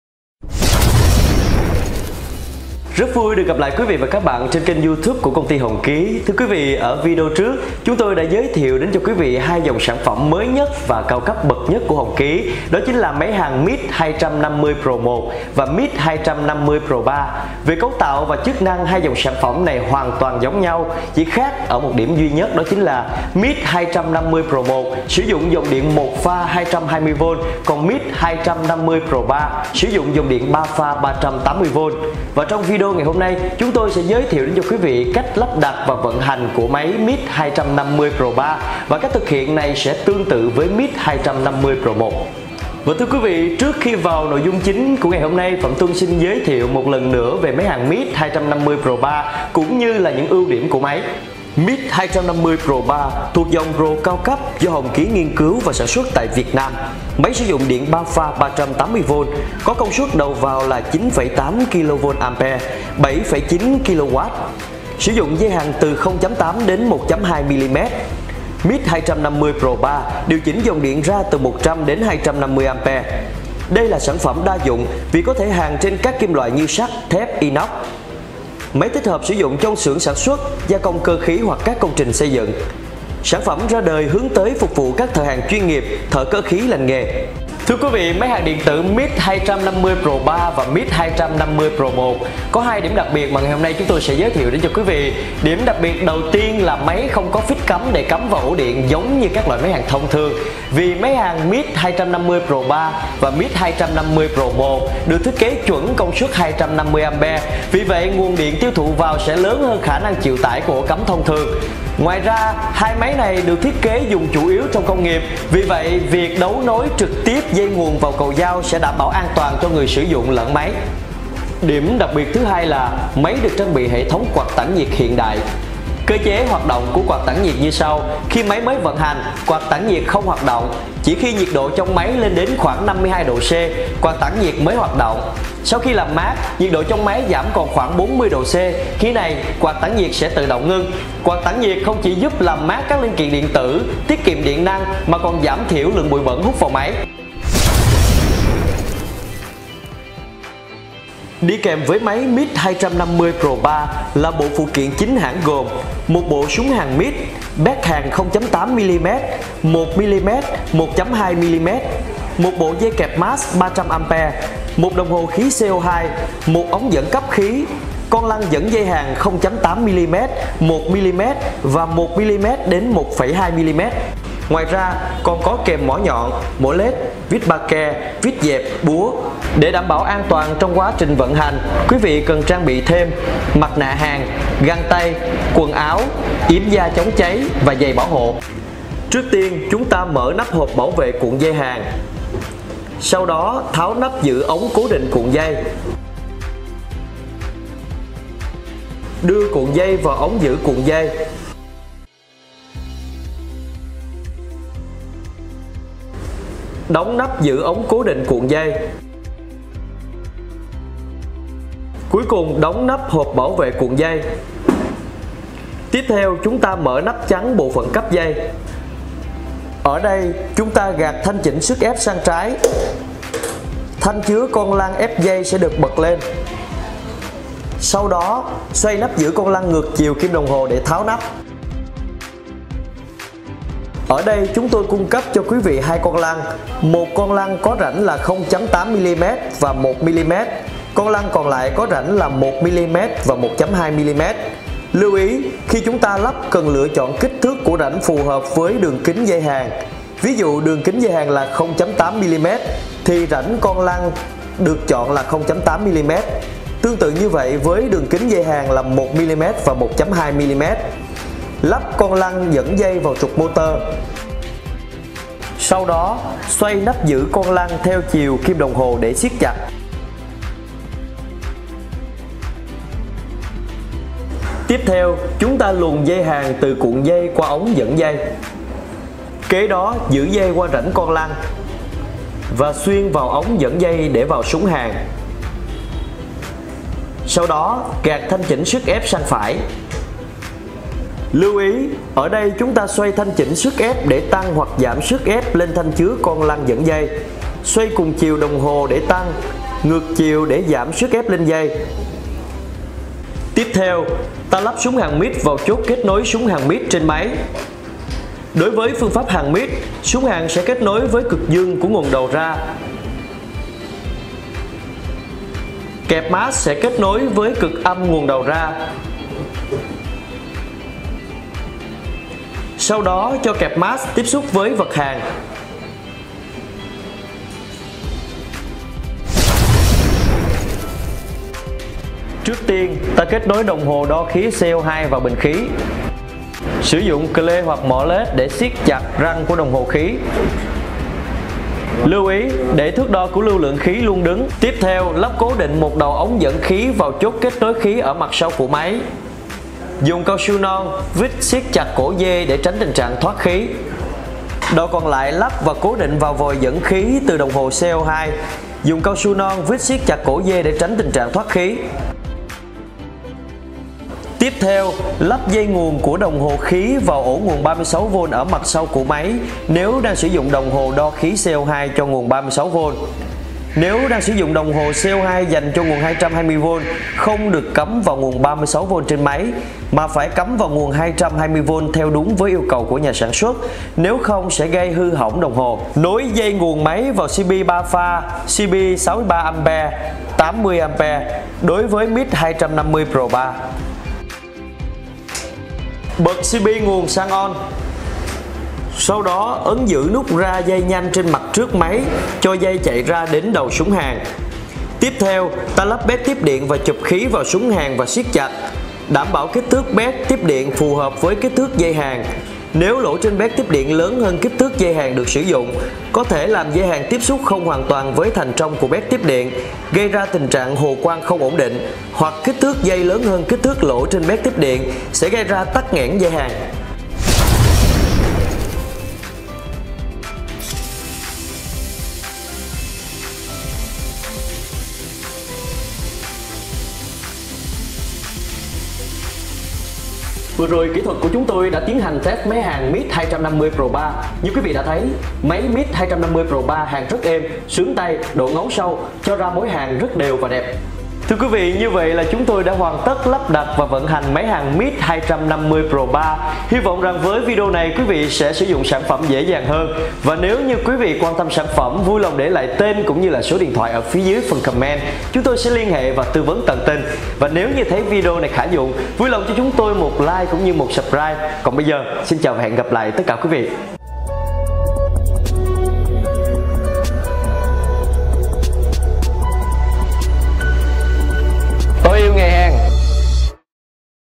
Máy hàn điện tử MIG 250 PRO3 - Made in Vietnam| Hướng dẫn chi tiết cách lắp ráp, sử dụng
Nhằm tối ưu hóa những hạn chế của các dòng máy hàn điện tử đang có trên thị trường, Hồng Ký đã nghiên cứu, phát triển và cho ra đời thế hệ máy hàn điện tử Mig 250 Pro 3 với nhiều ưu điểm vượt trội.

Công suất mạnh mẽ:
Mig 250 Pro 3 thuôc dòng Pro cao cấp, sử dụng nguồn điện 3 pha 380V ứng dụng công nghệ INVERTER. Máy hàn điện tử Mig 250 Pro 3 có công suất đầu vào 9.8 KVA, 7.9 KW mạnh mẽ cho phép làm việc hiệu quả, nhanh chóng, sử dụng cuộn dây từ 0.8 – 1.2 mm và bộ cấp dây bên trong máy. Điều chỉnh dòng điện ra từ 100 – 250A. Hiệu suất làm việc trung bình đạt 60%.

Máy hàn điện tử Mig 250 Pro 3 là sản phẩm đa dụng vì có thể hàn trên các vật liệu inox, sắt, thép, nhôm… cho mối hàn tuyệt đẹp với thời gian hàn không giới hạn, mồi lửa nhạy và hệ thống dẫn nhiệt cực tốt.

Thiết kế hiện đại, tiện lợi:
Với trọng lượng máy 27kg, thiết kế mạnh mẽ, chắc chắn, thuận tiện cho người sử dụng dễ dàng thao tác, vận chuyển trong nhiều điều kiện làm việc khác nhau. Chức năng hiển thị dòng hàn, điện áp hàn bằng bằng đồng hồ kỹ thuật số giúp người dùng thuận tiện theo dõi mức độ làm việc của máy và dễ dàng điều chỉnh.

Tiết kiệm và an toàn:
Một điểm nổi bật của máy hàn Mig 250 Pro 3 khiến nhiều khách hàng lựa chọn là máy có thể hoạt động ổn định cả khi điện yếu (lớn hơn 180V). Ứng dụng tính ưu việt của công nghệ INVERTER nên máy có khả năng tiết kiệm năng lượng đến 50% – 60% điện năng so với máy hàn que truyền thống. Máy hàn điện tử Mig 250 Pro 3 còn có chế độ bảo vệ (tự ngắt) khi quá nhiệt, quá tải hoặc nguồn điện không ổn định giúp máy làm việc bền bỉ hơn.

Mig 250 Pro 3 hiện là sự lựa chọn hàng đầu của thợ chuyên nghiệp tin dùng các sản phẩm Hồng Ký. Với hiệu suất cao, hoạt động ổn định, sở hữu nhiều tính năng vượt trội - Mig 250 Pro 3 hứa hẹn là bạn đồng hành tin cậy cùng thợ hàn chinh phục đỉnh cao sự nghiệp.

Địa chỉ: 38 Tây Lân, phường Bình Trị Đông A, quận Bình Tân, TPHCM